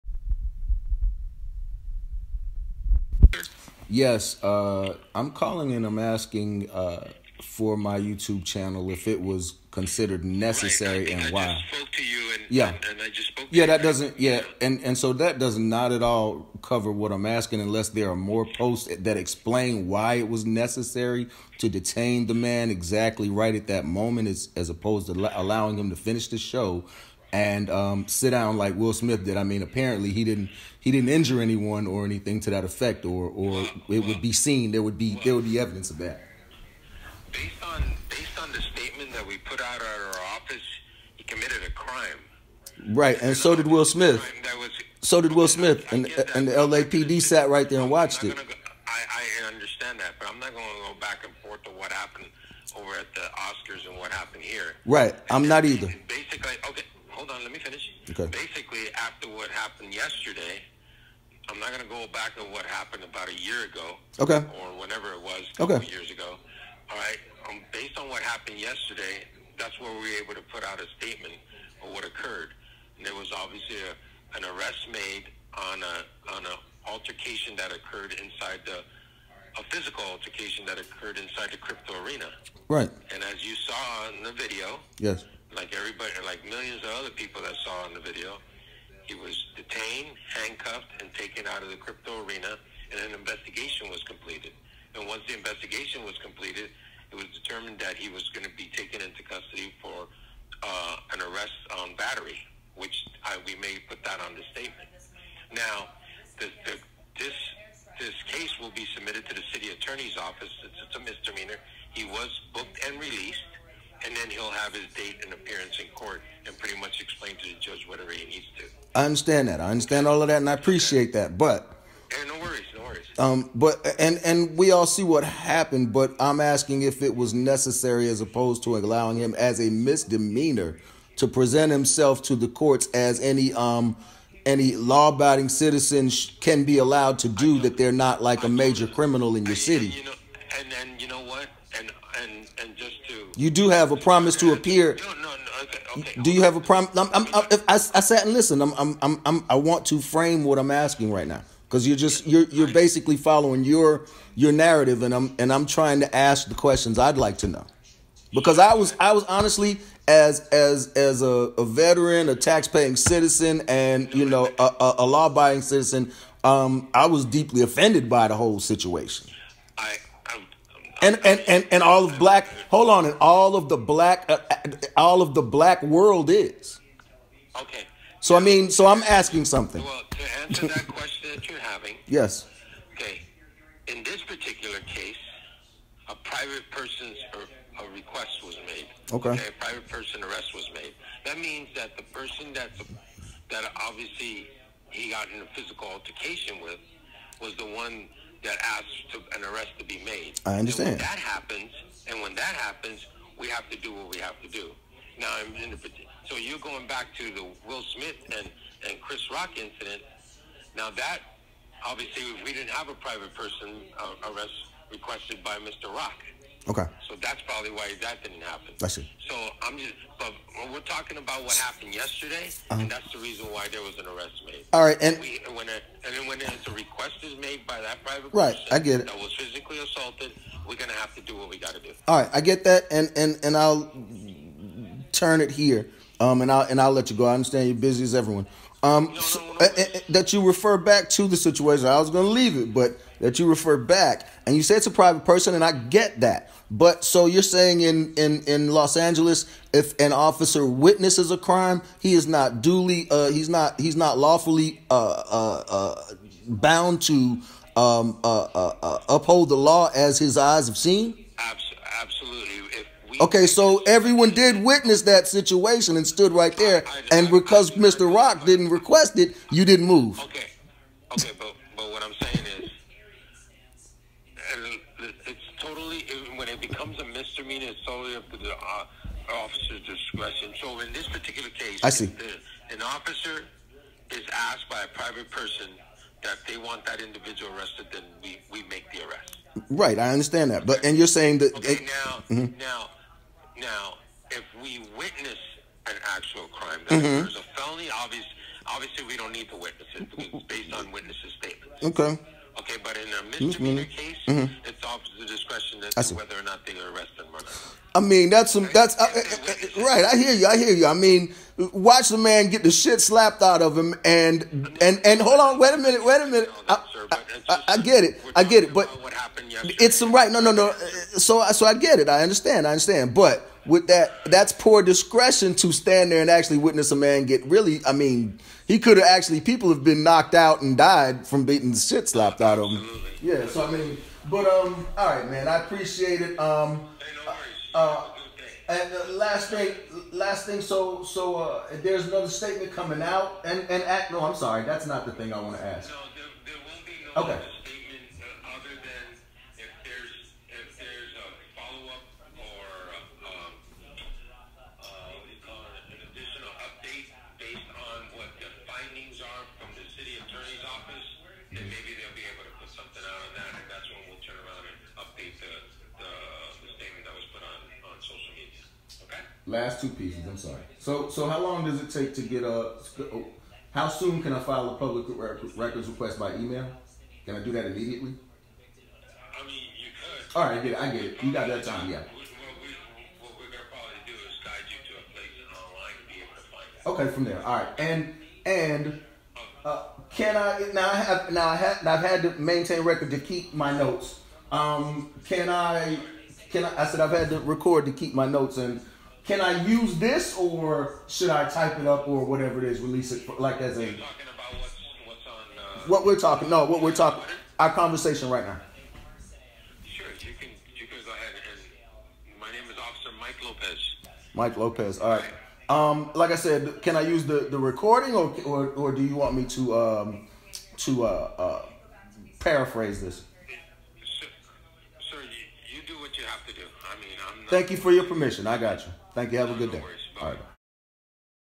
LAPDPIO Ofc. Mike Lopez with Jaw-Dropping Interview On KILLER MIKE's Arrest at the GRAMMYS
In a phone call with the LAPDPIO questions that everyone wants to hear answers to.